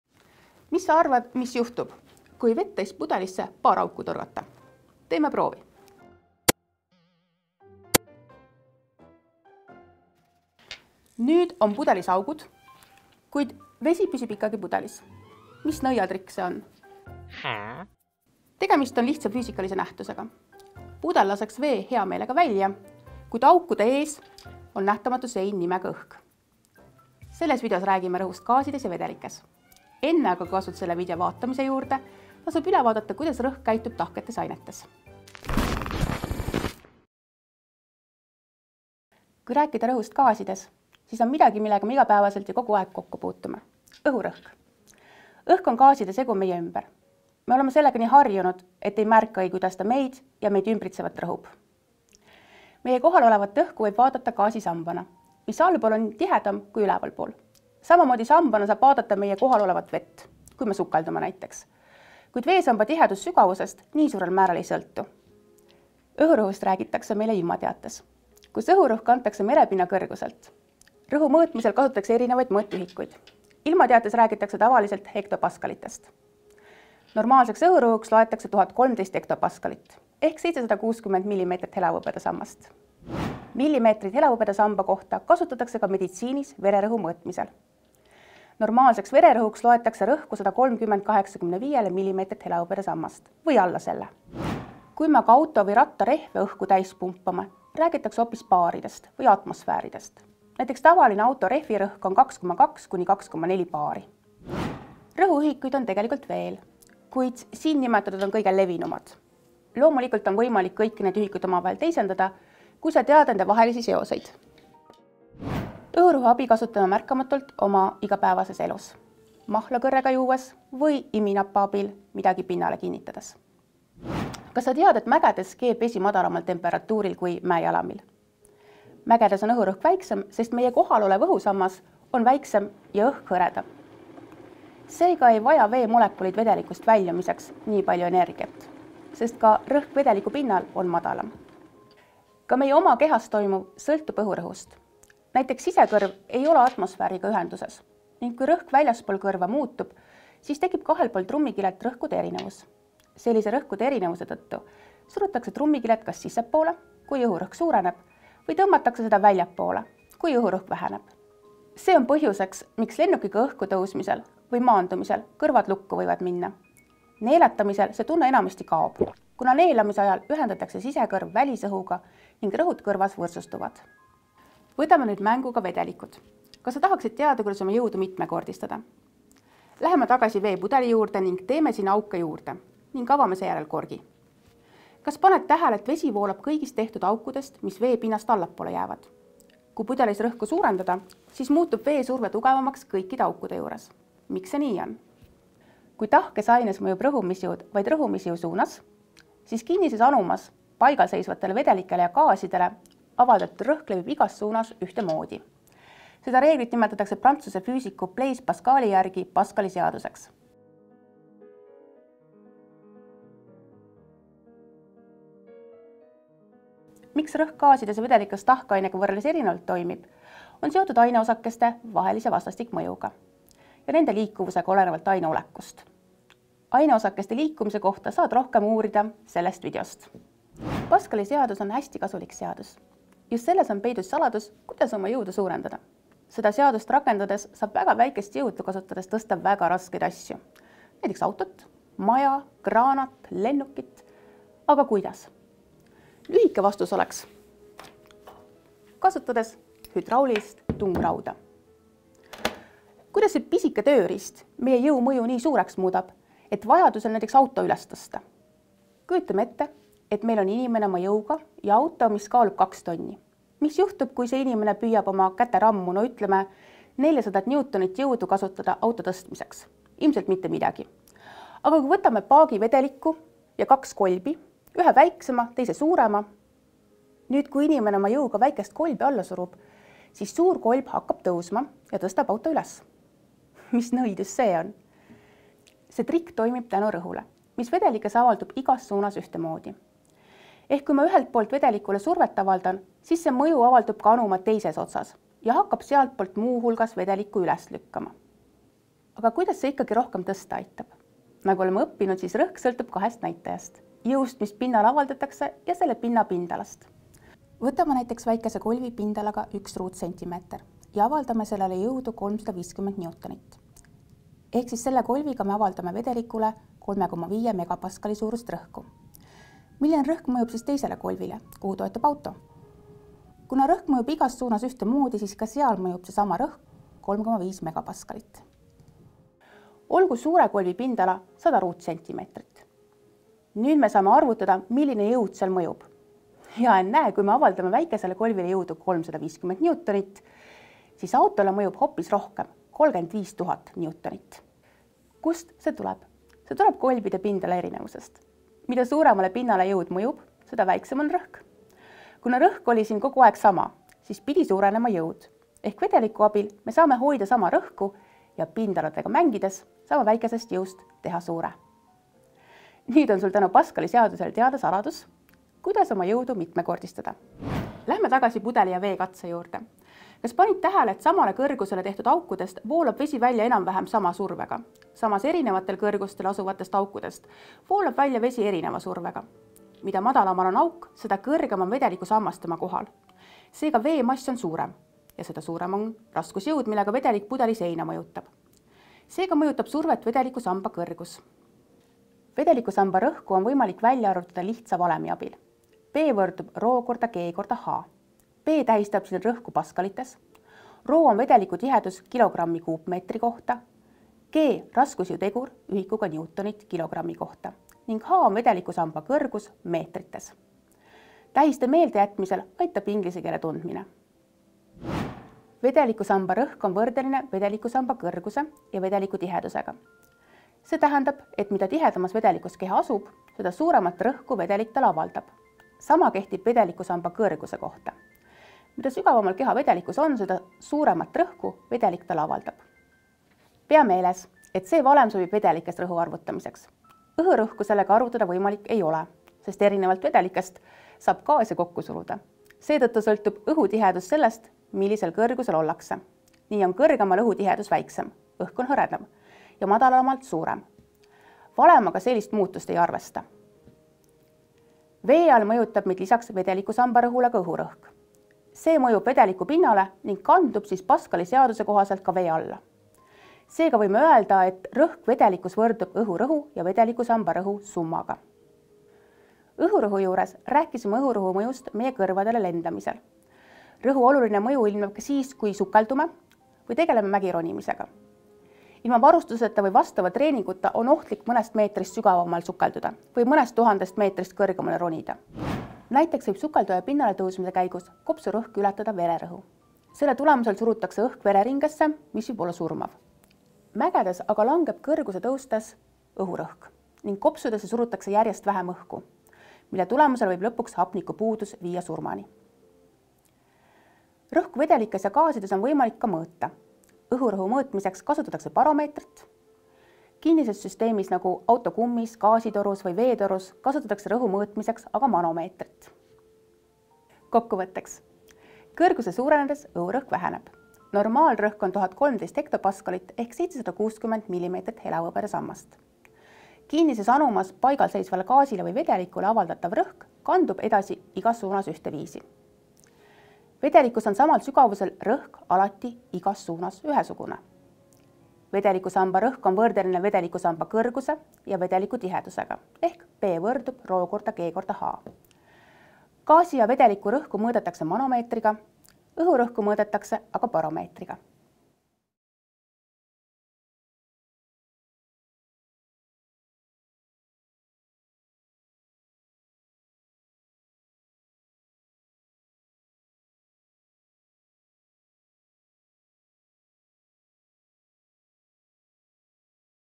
Rõhk vedelikes ja gaasides | VIDEOÕPS Füüsika. Mehaanika #6
Mehaanika seeria 6. videos tutvustab Hele-Riin, kuidas toimib rõhk vedelikes ja gaasides. Käsitletakse mõisteid: õhurõhk, vedelikusammas, gaasisammas, hektopaskal, mmHg, normaalne õhurõhk, normaalne vererõhk, tavaline rehvirõhk, atmosfäär (ühik), baar (ühik), sisekõrv, väliskõrv, Pascali seadus, hüdrauliline tungraud, baromeeter, manomeeter.

Video on mõeldud kasutamiseks põhikooli III kooliastme füüsika õppes, kus teemaks on mehaanika.

Sisukord:
00:00 Sissejuhatus
00:39 Õhurõhk
01:23 Vererõhk
01:34 Rehvirõhk
01:45 Rõhuühikud
02:04 Õhurõhk mägedes
02:22 Miks kõrvad lukku lähevad?
02:55 Katse pudeliga
03:16 Pascali seadus
03:51 Hüdraulika tööpõhimõte
04:20 Animatsioon
06:07 Vedelikusamba kõrgus. Pudeli katse seletus
06:32 Vedelikusamba rõhu valem
07:34 Rõhk vedelikus
07:49 Ohud sukeldumisel ja mägironimisel
08:24 Baromeeter ja manomeeter
08:36 Kokkuvõte

Video on valminud heade Eesti ettevõtete toel, Lae End programmi raames.

Video sisu: Hele-Riin Kutsar, Kristo Siig, Ingrid Rõigas
Kaamera ees: Hele-Riin Kutsar
Filmimine: Martin Männik
Montaaž: Anders Aruoja
Režissöör ja projektijuht: Kristo Siig

Videole on omistatud CC-BY autoriõiguste litsents, mis võimaldab materjali vabalt taaskasutada, kui viidatakse autorile (MTÜ Videoõps, Praktikal Education OÜ ja SA Enefit Tõukefond).